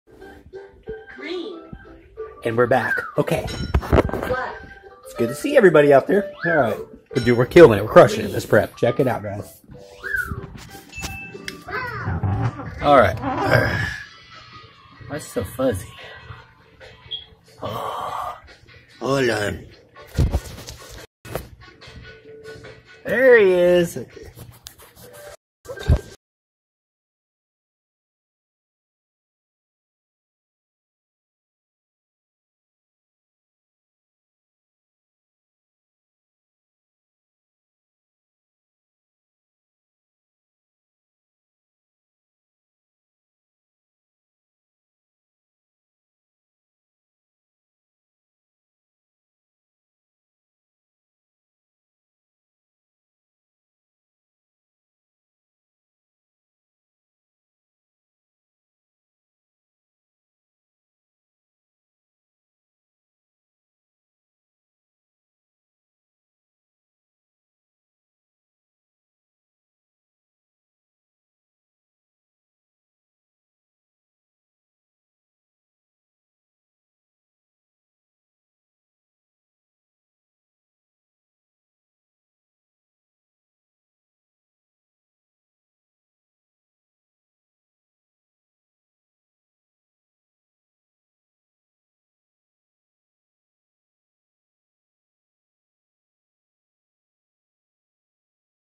God-lord Series Day 2 - Foods For Cutting Weight
A few samples of great food options to lose fat and trim down! Cutting baby!